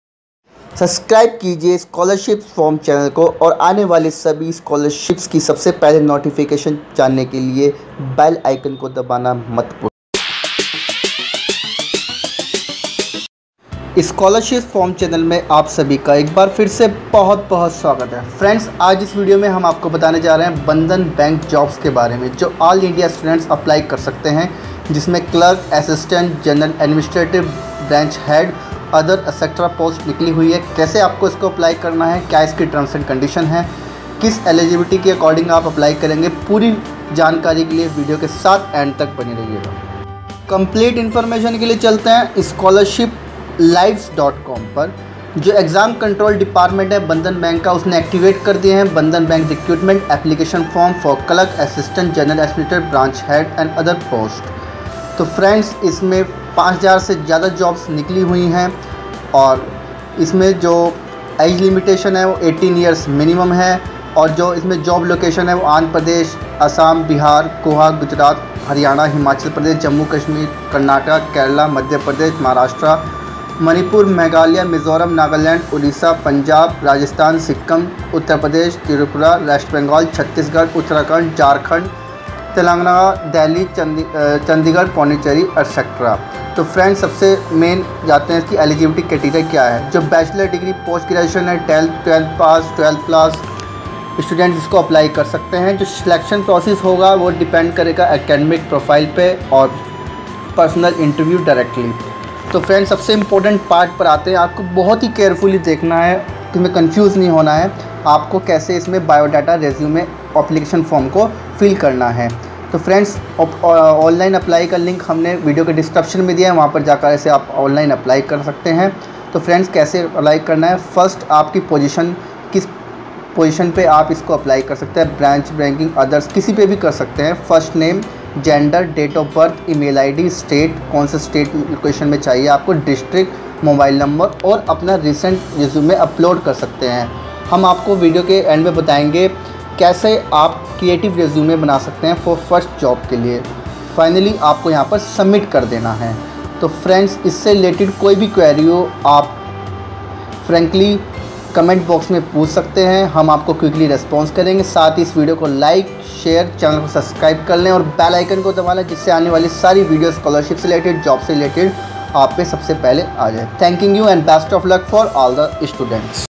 Bandhan Bank Recruitment 2018 5000 Clerk/Officer Vacancies
Bandhan Bank has activated Bandhan Bank Recruitment 2018 Application form for Clerk, Assistant, General Administrative, Branch Head